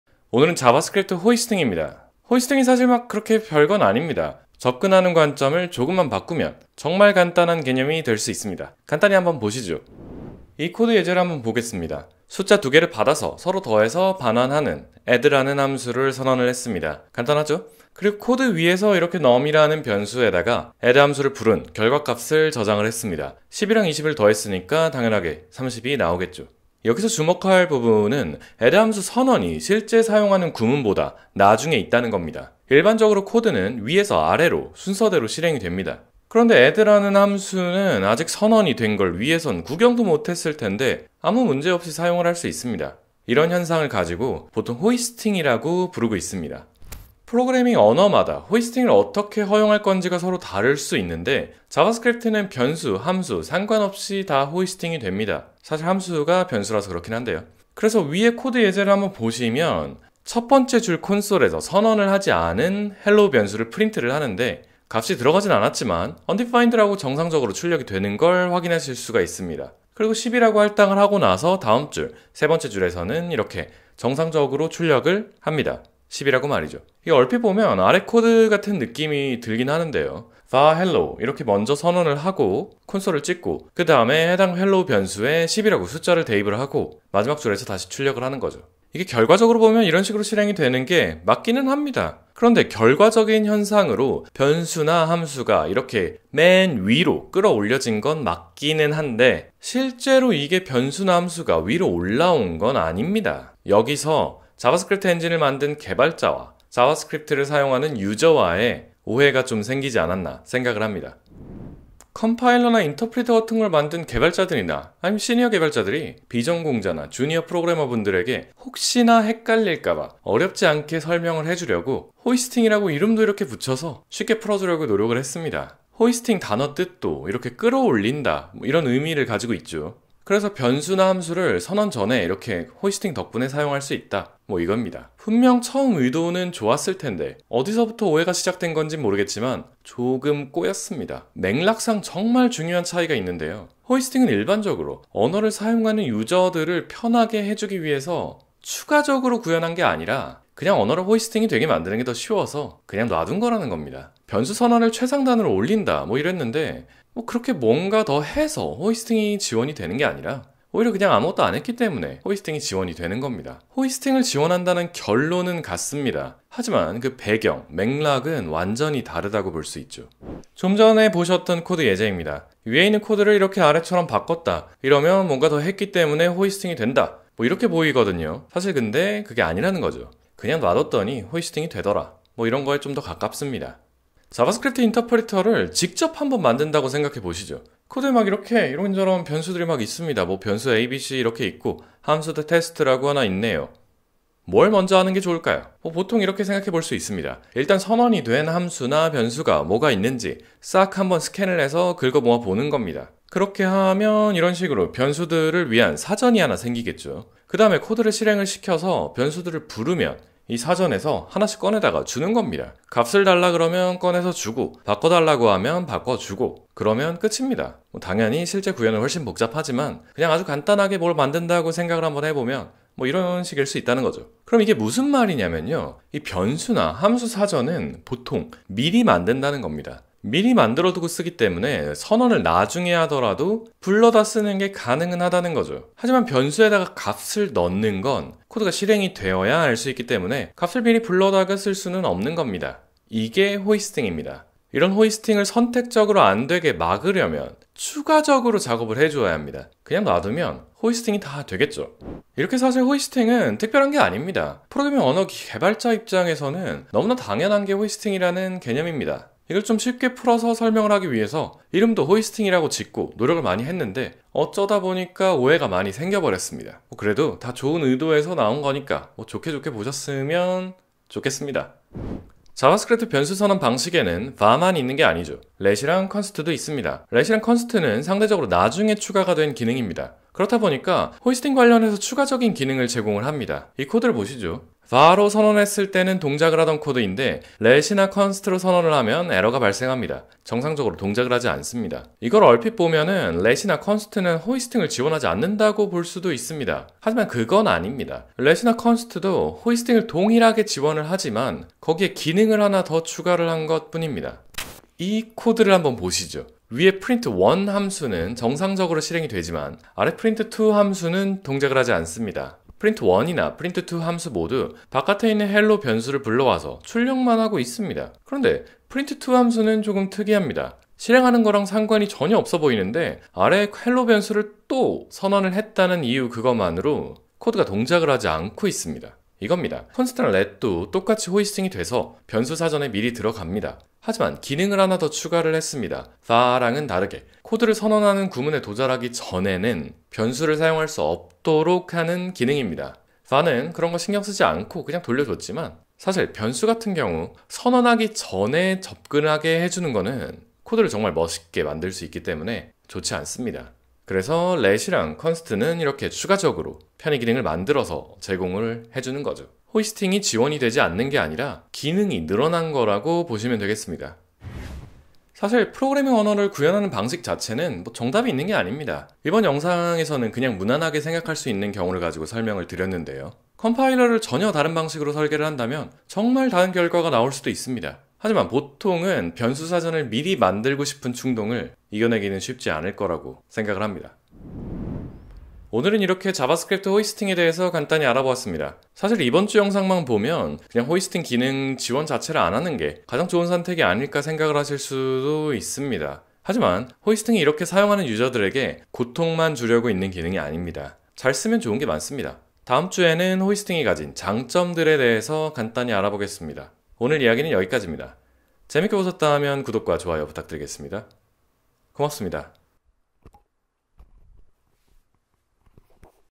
자바스크립트 호이스팅, 흔한 오해 1가지
자바스크립트 호이스팅에 대해서 간단히 알아보겠습니다.